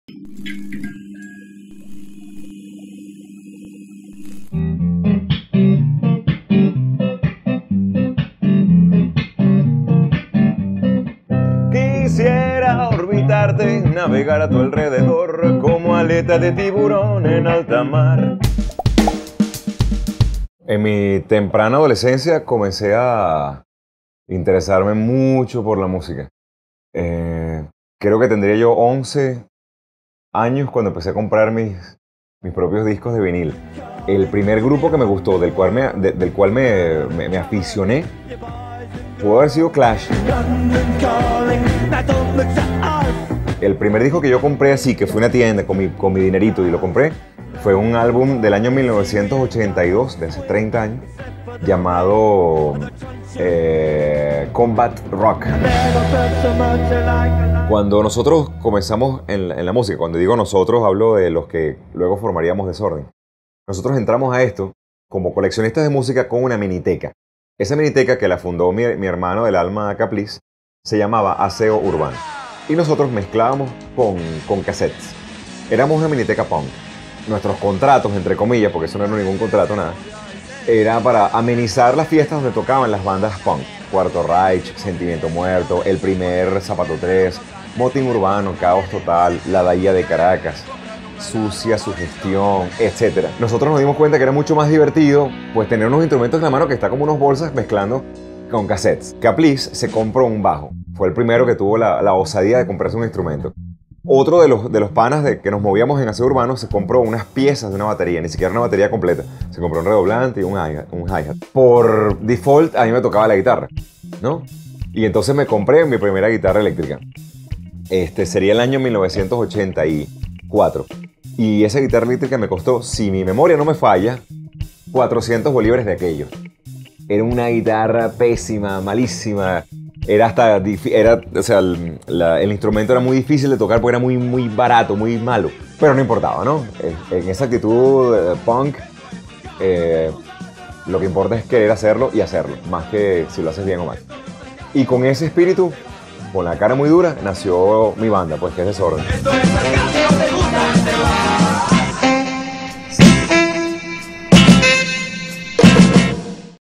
-- PopUp -- Horacio Blanco En Su Casa Pt.1 (CochinoPop.com)
// Horacio Blanco, nos cuenta como empezó en la música, los inicios de Desorden Público y su amor por la salsa.